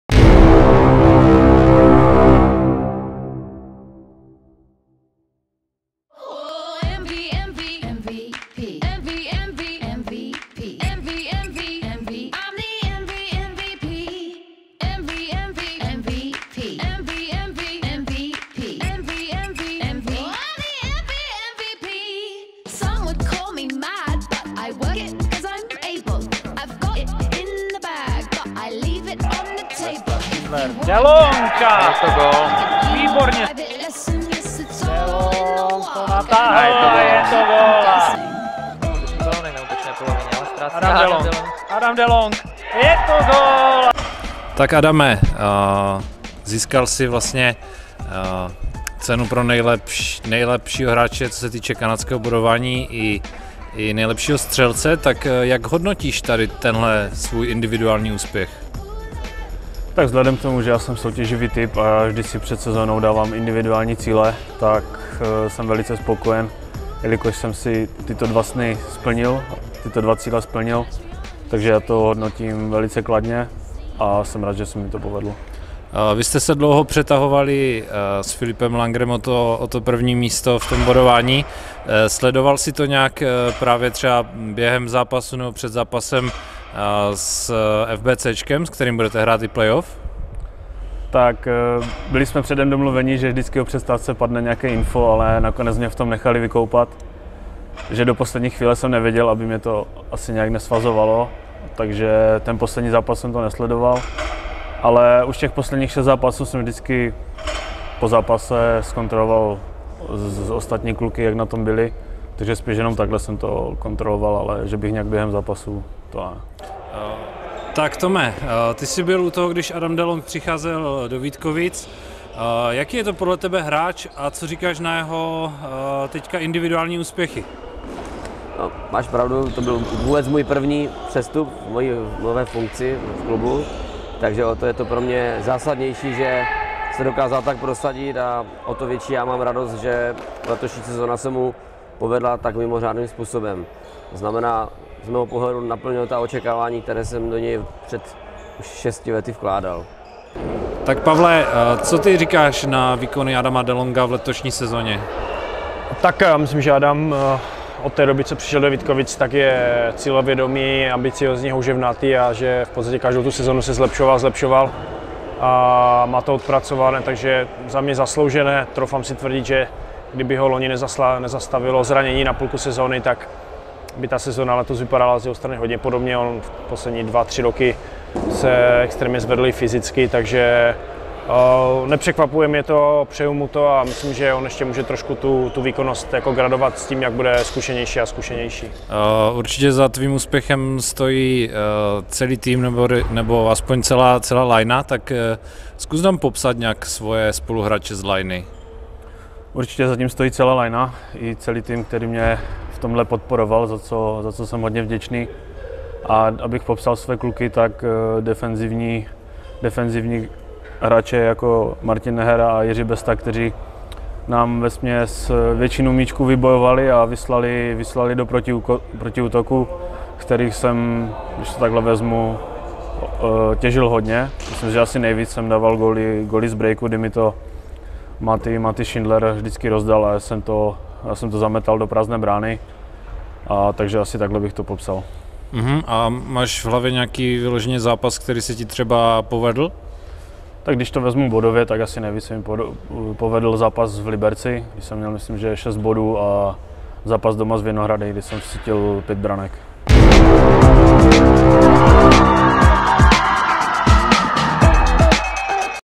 PLAY OFF 2021 | DELI TOMU VELÍ
Adam Delong, nejproduktivnější hráč a nejlepší střelec základní části 2021 LIVESPORT Superligy.